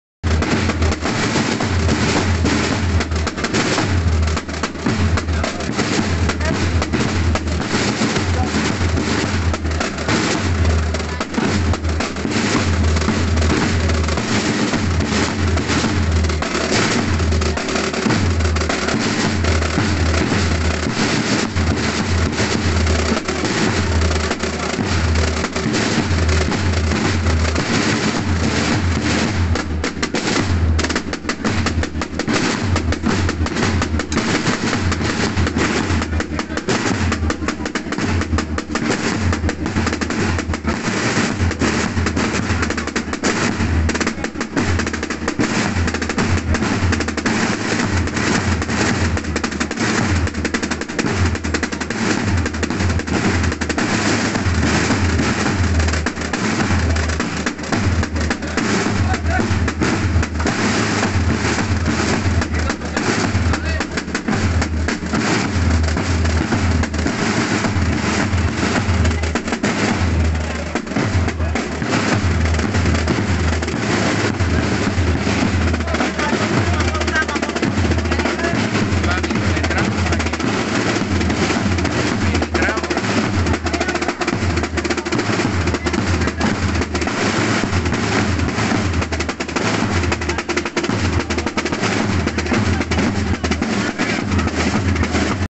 Revirá de San Bartolomé, Patrón de Nerva - 2024
Un año más, San Bartolomé procesionó por las calles de su pueblo minero de Nerva (Huelva) en un festivo 24 de Agosto.